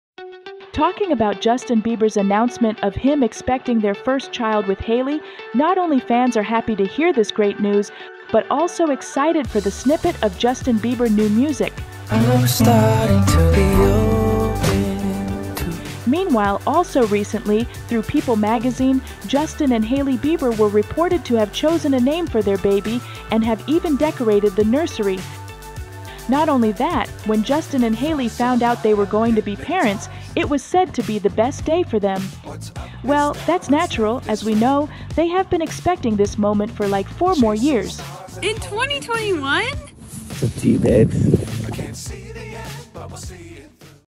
Justin and Hailey Bieber have chosen a name for their baby, have even decorated the nursery??!
Talking about Justin Bieber’s announcement of him expecting their first child with Hailey, not only fans are happy to hear this great news, but also excited for the snippet of Justin Bieber new music.

Meanwhile, also recently, through PEOPLE magazine, Justin and Hailey Bieber were reported to have chosen a name for their baby and have even decorated the nursery. Not only that, when Justin and Hailey found out they were going to be parents, it was said to be the best day for them.

Well, that's natural, as we know, they have been expecting this moment for like 4 more years.